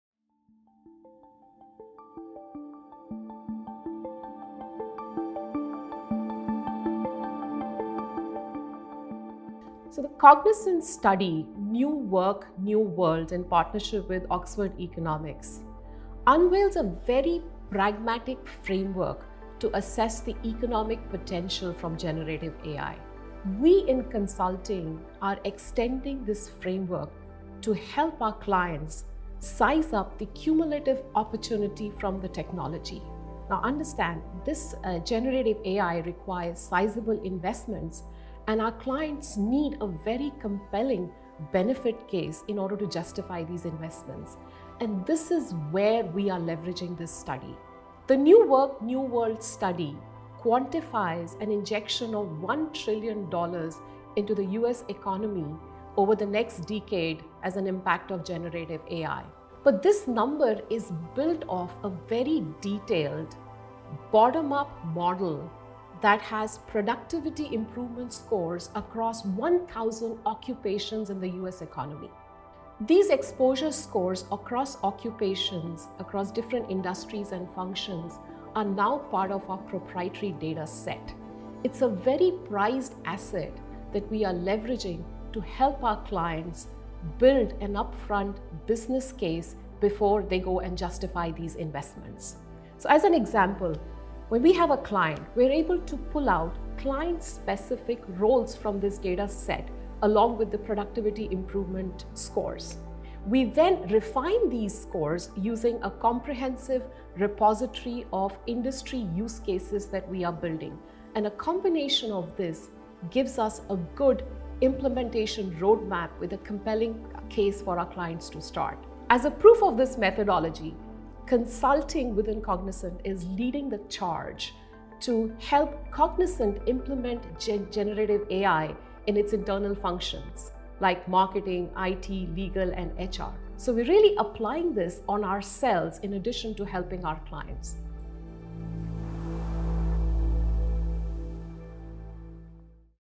Research Report Sheds Light on the Economic Impact of AI | Shveta Arora | Cognizant
The study quantifies an injection of $1 trillion into the US economy over the next decade, impacting occupations, across different industries and functions ranging from marketing, legal, IT and HR. Join Cognizant’s Shveta Arora, SVP and Global Head of Consulting and Client Experience for an overview of this report and findings.